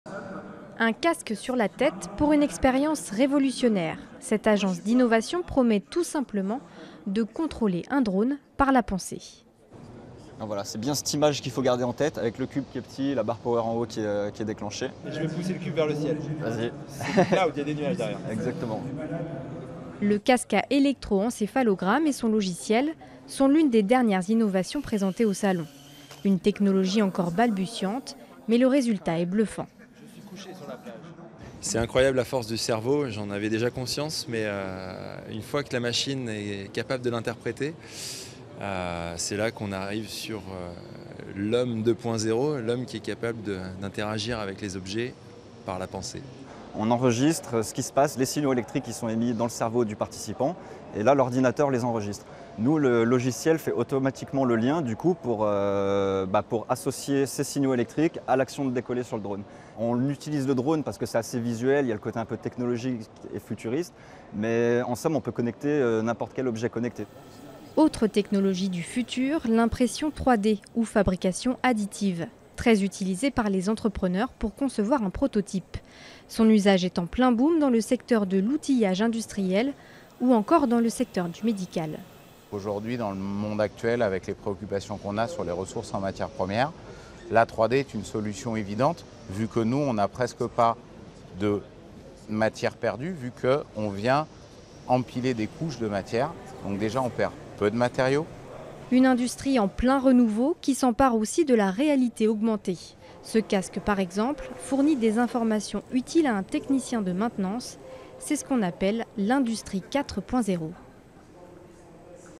TITRE INFORMATIF - EXPRESSION INCITATIVE
En Savoie, dès jeudi 6 décembre 2018 à la halle olympique d'Albertville, le salon Alpes Tech se déroule sur 3 jours. Réalité virtuelle, impression 3D... Ces technologies du futur sont très prisées par une industrie en plein renouveau, l'industrie 4.0

Intervenants : Guillaume Juan (Cofondateur Kakuz), Mohamed Menouer (Responsable commercial Delta 3D Print)

Reportage : M.Feutry, M.Quemener, Montage : L.Bouchaud

Retrouvez-nous sur :
Notre site web